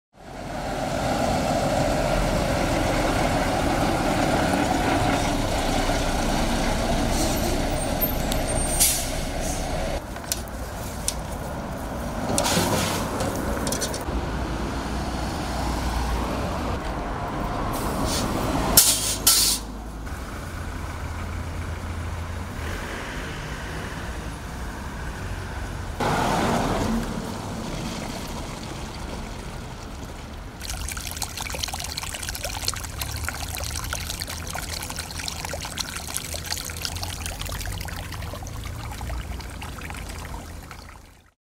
Hasiči pomáhají v Hradci Králové zalévat městskou zeleň
Dnes se k vozům technických služeb, které průběžně už několik měsíců zalévají ve městě zeleň, připojily také cisternové vozy městských dobrovolných hasičů.